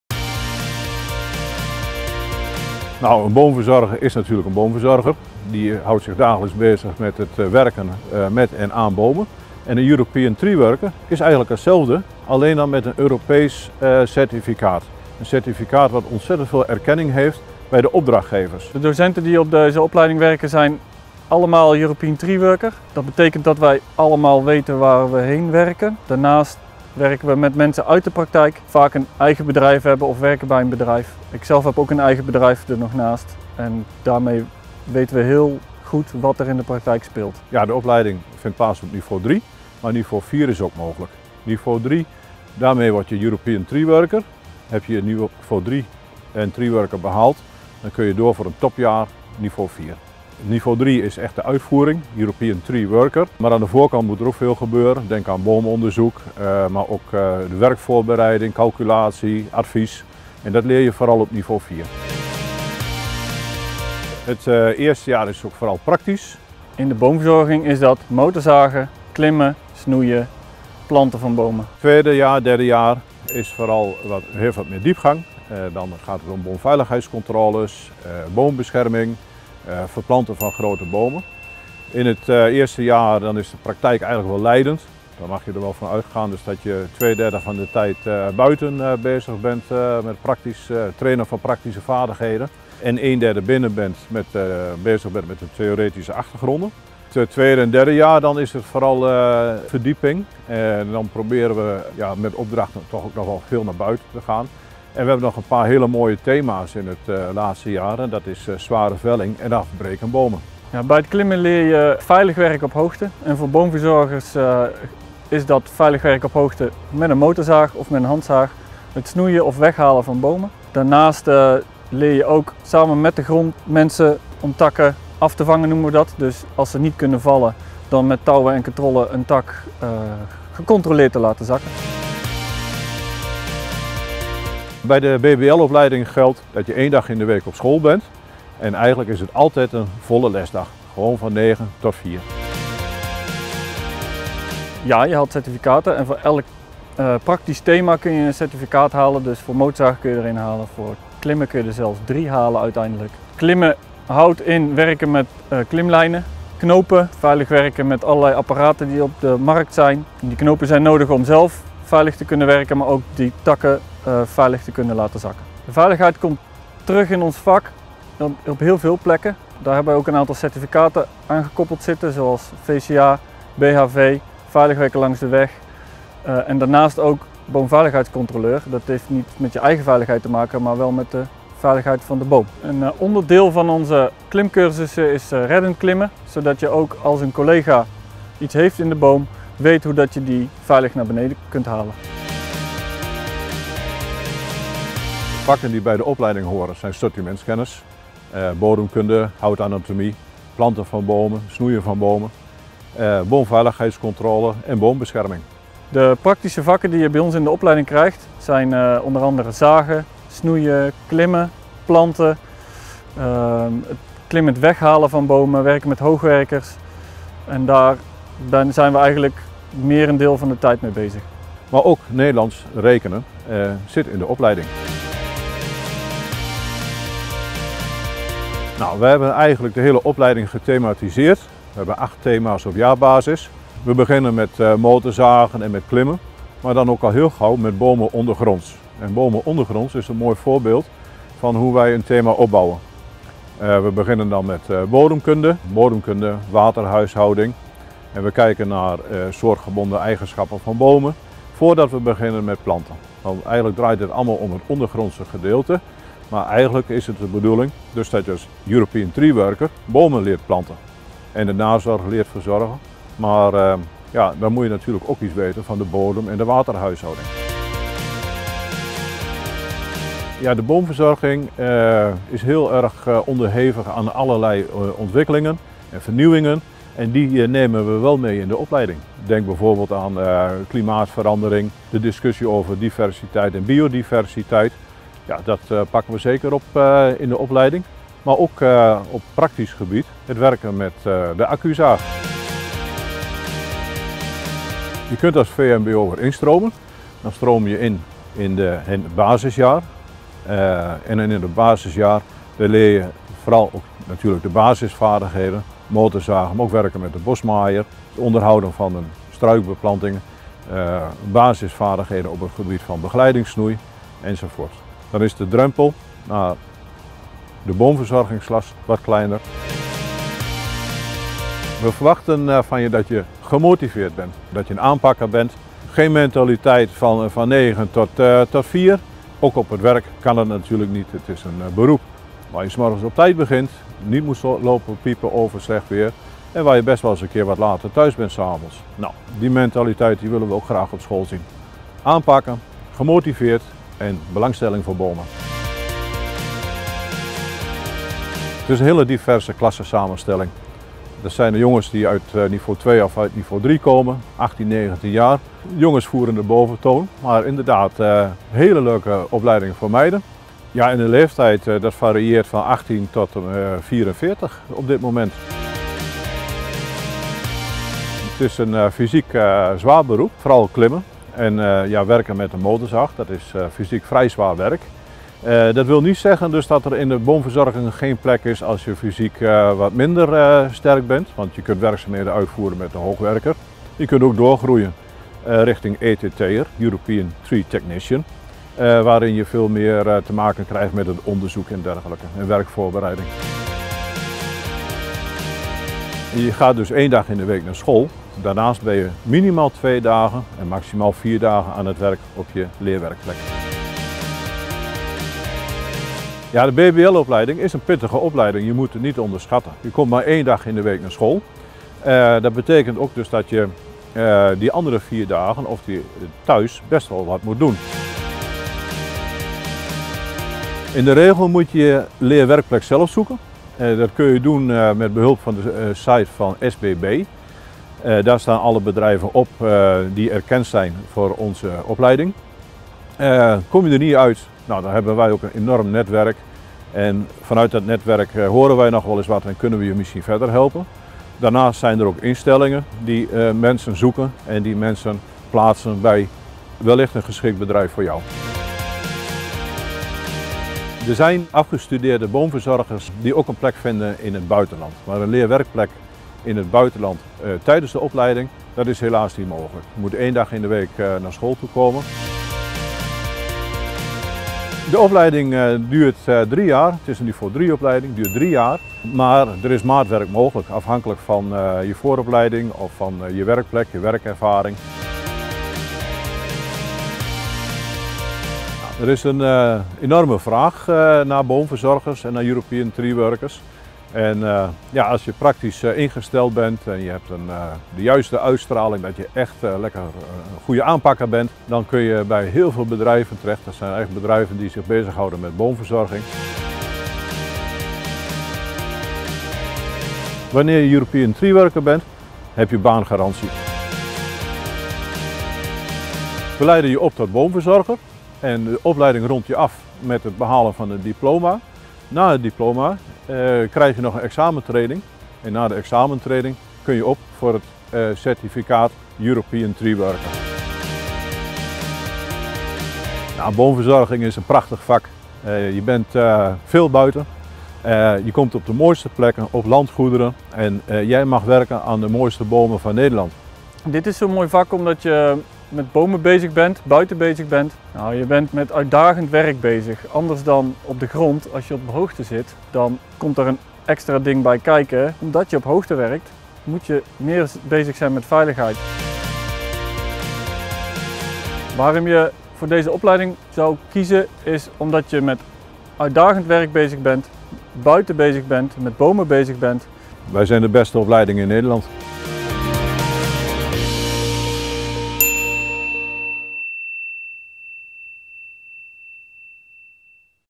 Boomverzorging ETW
Wil jij weten hoe je een boom van duizenden kilo’s verplant? Of leren hoe je met klimuitrusting en motorkettingzaag boven in een grote boom aan het werk kunt? Met de praktische BBL-opleiding Boomverzorging/ETW word je een echte bomenspecialist. Bekijk deze voorlichting voor meer info of check helicon.nl.